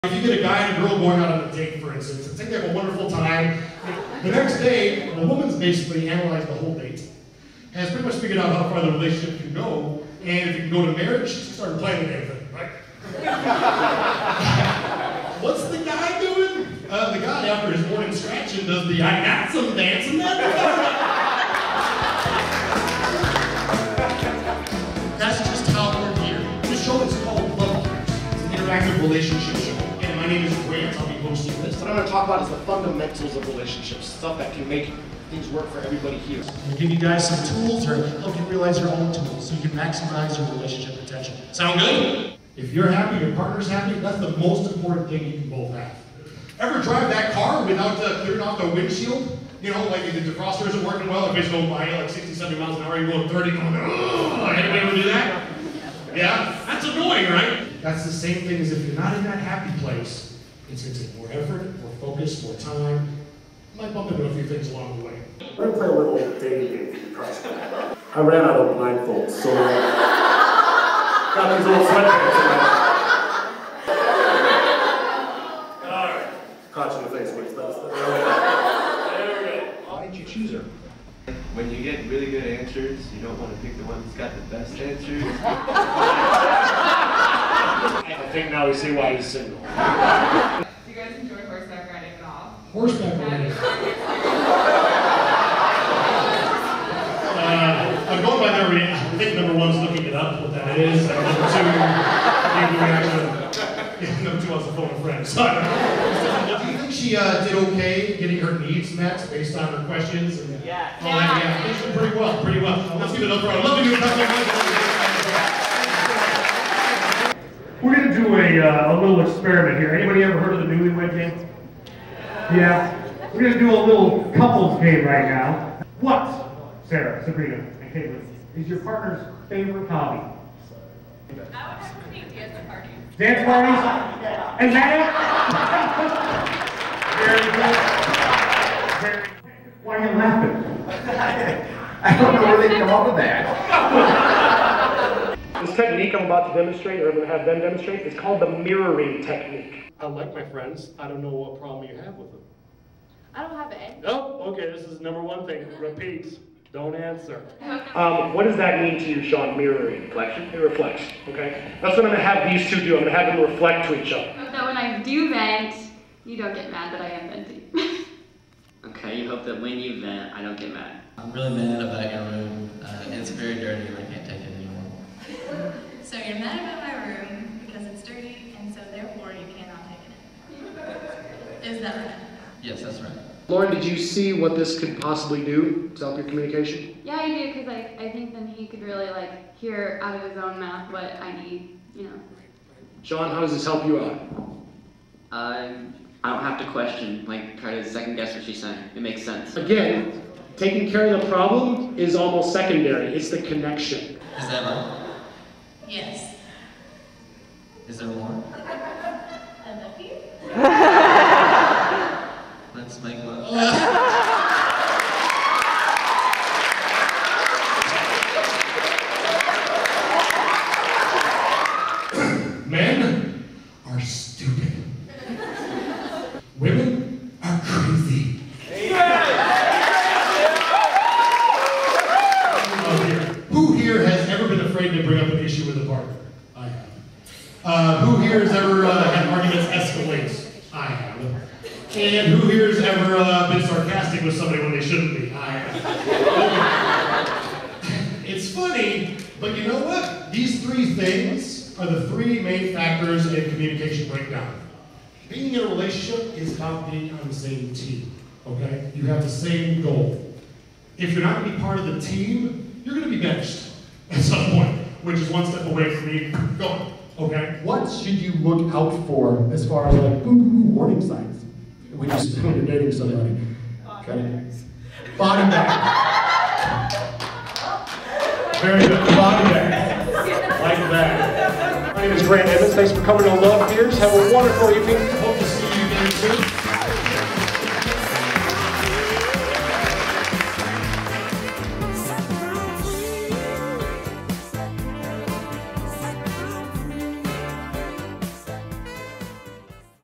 Grant Edmonds: Love G.E.A.R.S.
Grant Edmonds’ Activity Relationship Show (Love G.E.A.R.S.) is a humorous, interactive dating and relationship program, designed to help students create, maintain, and improve relationships of all types–from friendships and familial relationships, to the most intimate of relationships, including the one with yourself. While educational in nature, the program also includes many “inter-activities”: A dating game, a “couples” game (reminiscent of The Newlywed Game), and some hands’ on experience with healthy communication techniques. And it’s all done with a touch of humor sprinkled throughout! Learn the three major signs of communication breakdown and the ways to avoid them, find out exactly what qualities (in yourself and others) that you can live with, which ones you can’t, and come to understand that men are “dumb” and women are “crazy”… and learn to love them anyway! So gear up to learn how to improve your situation–whether you’re happily engaged, unhappily attached, or even if you’re single and couldn’t care less about marriage. There is something for everyone to learn! Enjoy laughing as you learn and emerge “geared up” to take on your past, current, and future relationships!

Grant Edmonds is the author of “Love GEARS: A Relationship Tool Book” and in his 30 years of existence, no one thing has been more important to him than the nature of relationships. Some of the concepts Grant teaches involve learning what type of soulmate fits you best, techniques to overcoming relationship crises, techniques for more effective communication, and most importantly, learning about who you really are and what you need from a partner and from yourself. Whether it’s dealing with friendships, romantic relationships, or just the relationship within yourself, the nature of human beings has always been his highest priority of study.  In Love GEARS, Grant shares his wisdom, experiences, and of course, humor…regarding anything relationship-worthy.

Love GEARS is the interactive, educational, comedic semi-lecture…with group therapy to boot!

Grant also hosts his Variety Game Show…featuring the other half of his personality…